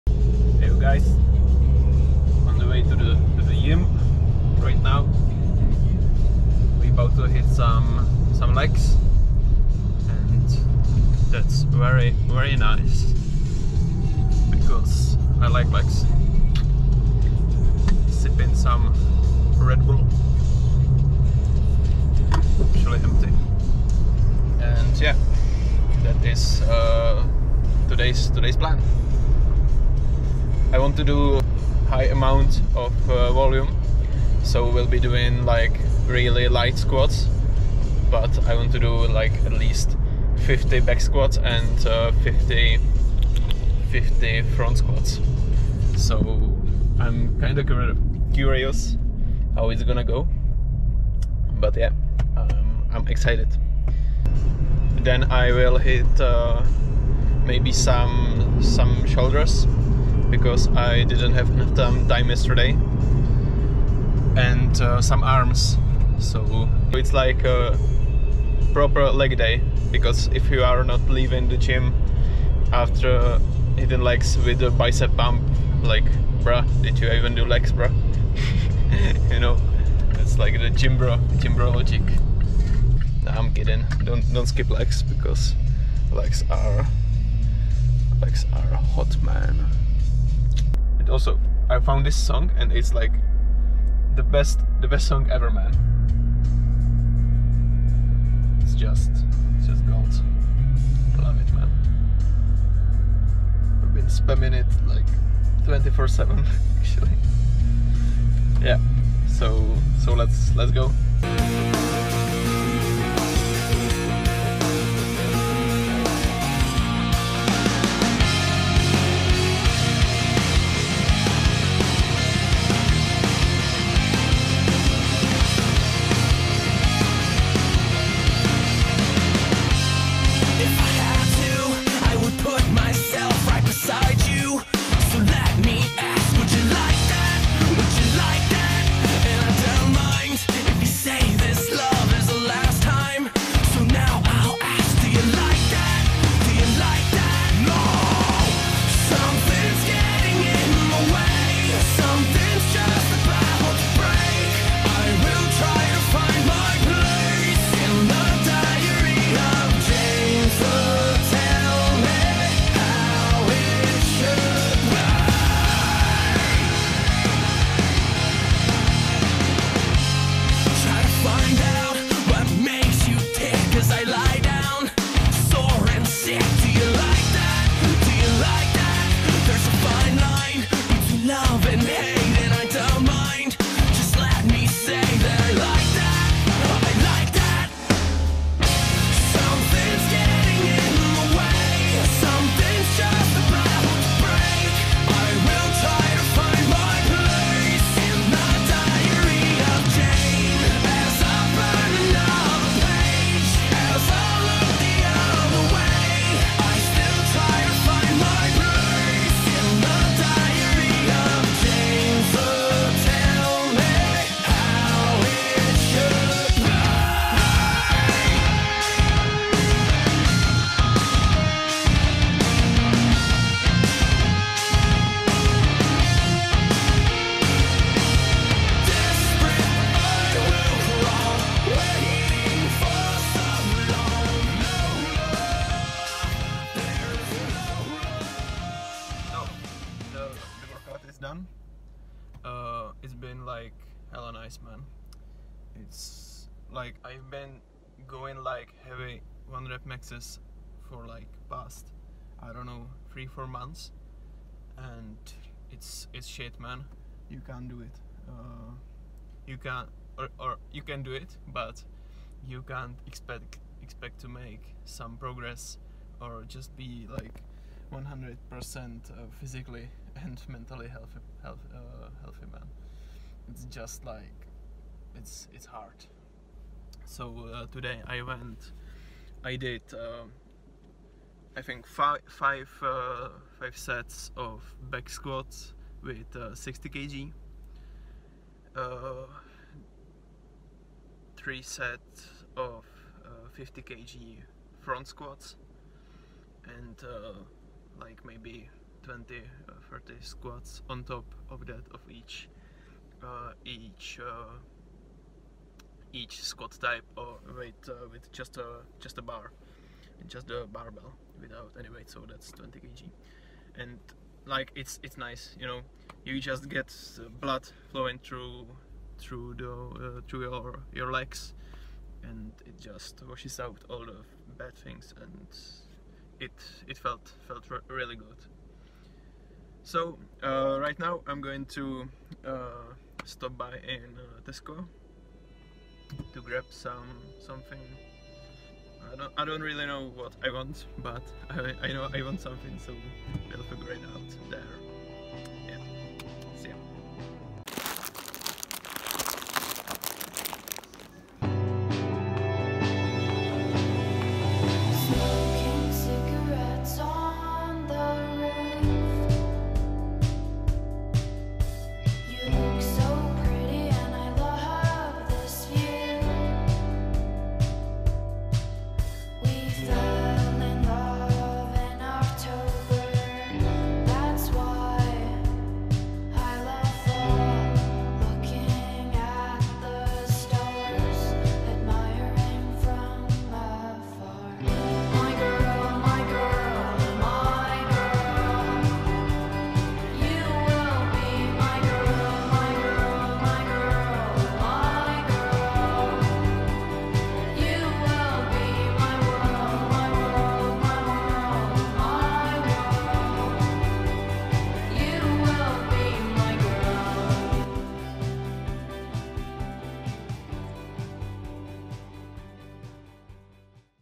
Squattingg and chillingg
Yeah, light squats today. I hope that they will be kinda restfull so I can continue in chasing new PR´s.